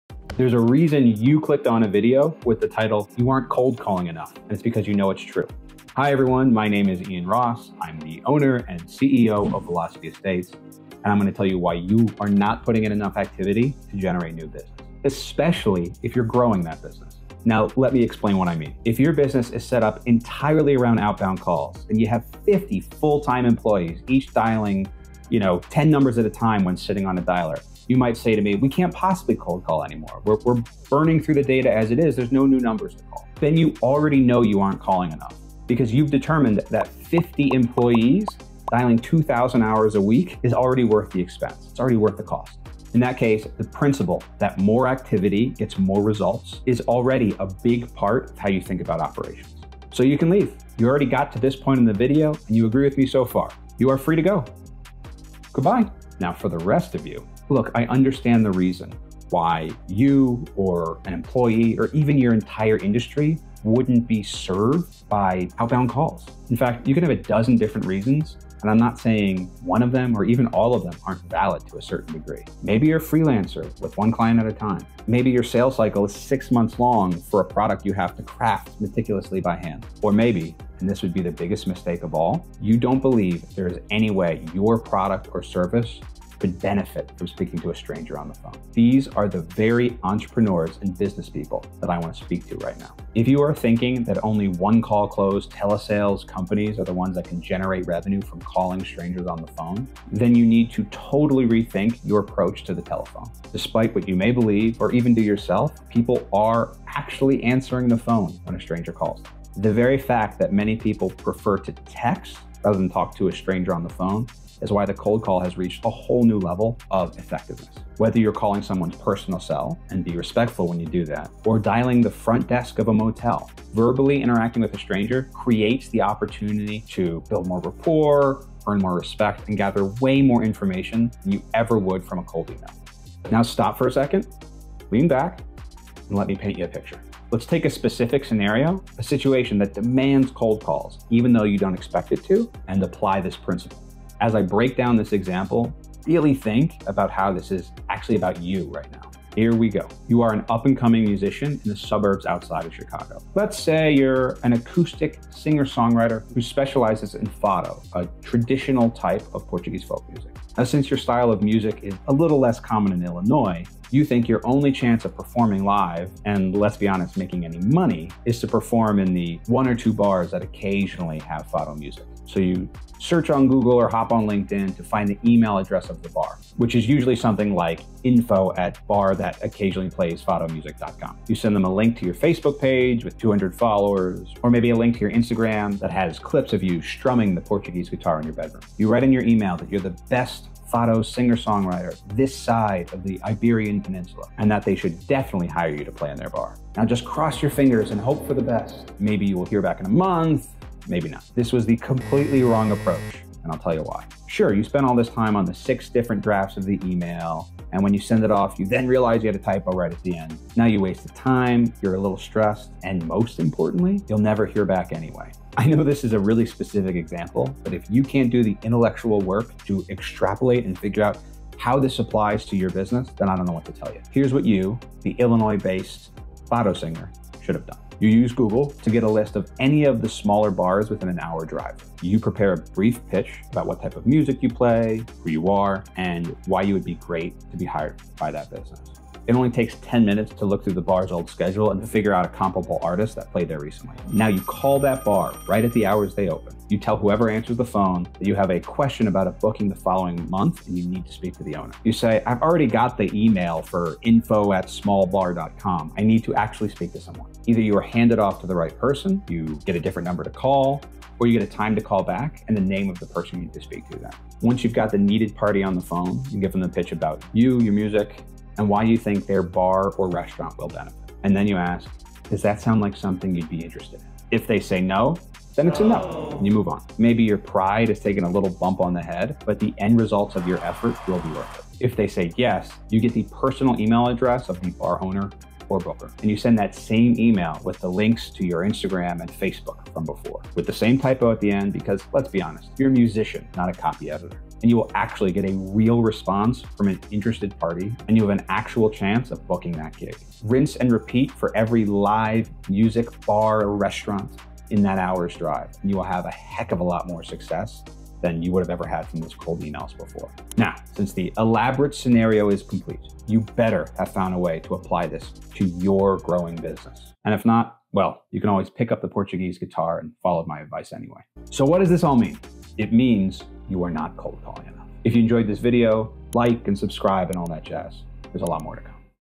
You Are Not Cold Calling Enough
Looking for a Sales Community? Visit: vividselling.co

Cold calling is one of the greatest ways to increase revenue and create opportunities. If you are an entrepreneur or business owner, chances are you are not spending enough time talking to strangers on the phone.

If you think that there's no way picking up the phone and dialing could help you grow your business, then this video is for you.